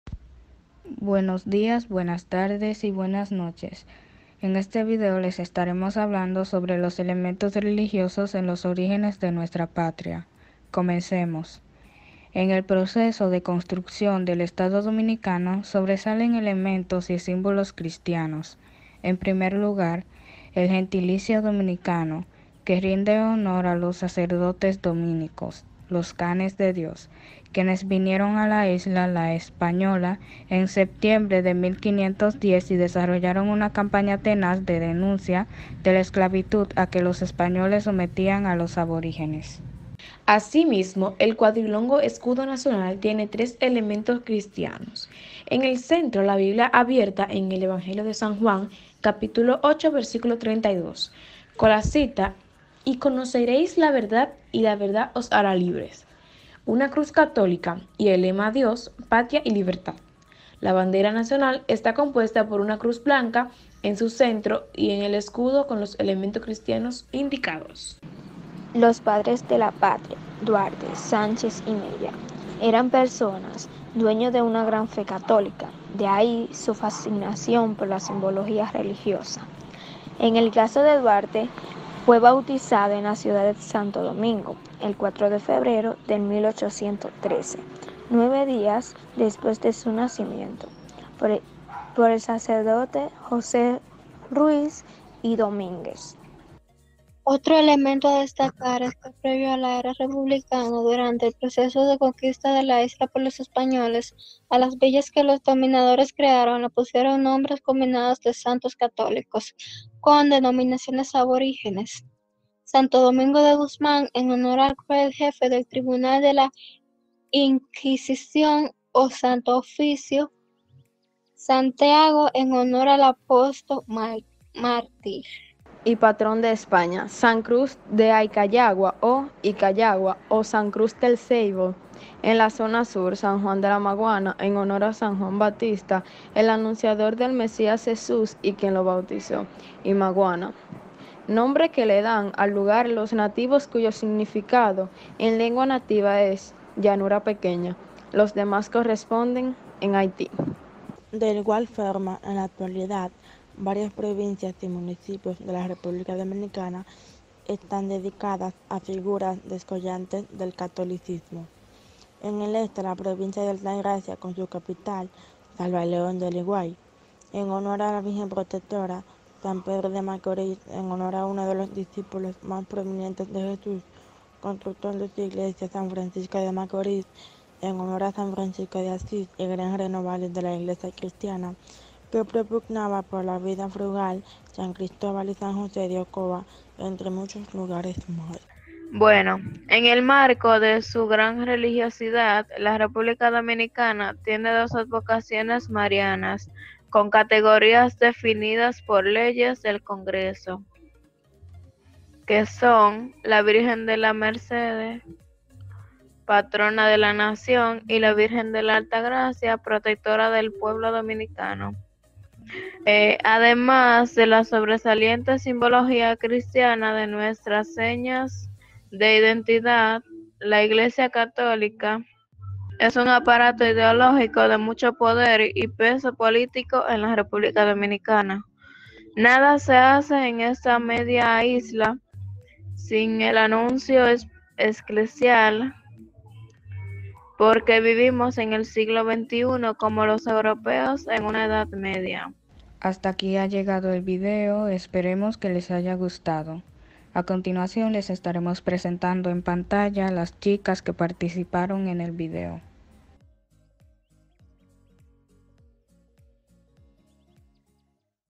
Trabajo de Formación Humana y Religiosa de 4to. DAAI
Trabajo de Formación Humana y Religiosa realizado  por nuestras  alumnas del PMMM con motivo de la celebración de nuestra Independencia  Nacional. Elaborado por 4to. grado. del área técnica Desarrollo y Administración de Aplicaciones Informáticas.